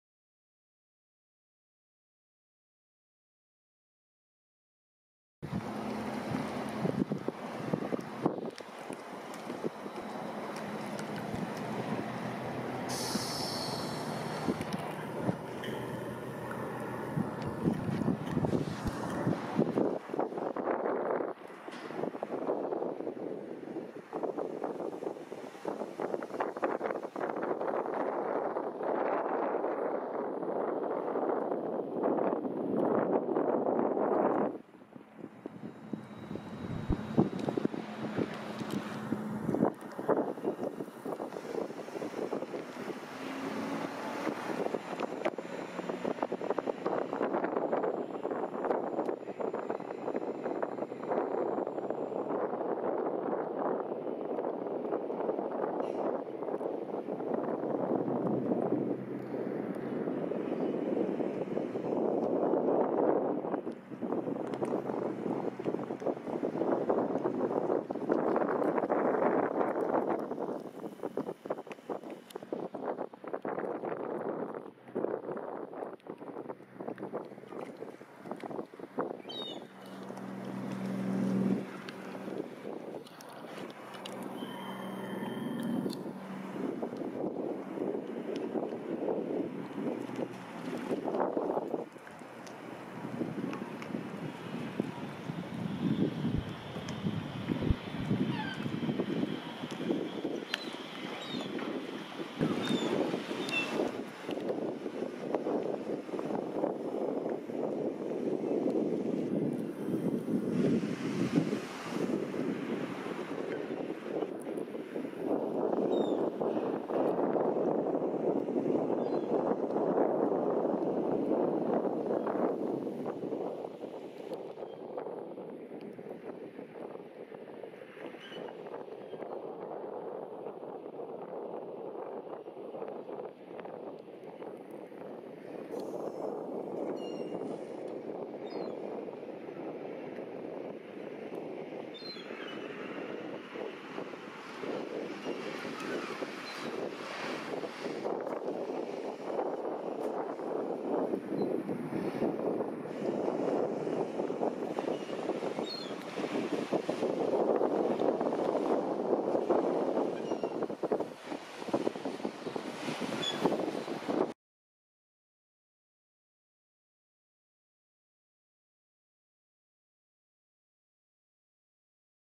朝8時30分の土生港の様子 令和2年1月20日
朝8時30分の土生港の様子です。
カモメが沢山いました。